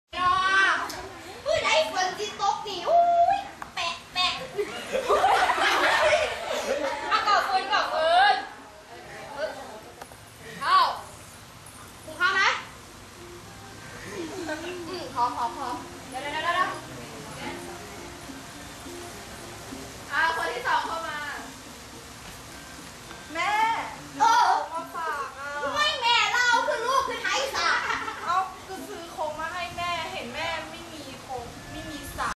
บทบาทสมมุติ
พี่สอนน้องในโครงการ "ยุวทูตวัฒนธรรมกับการละคร 53"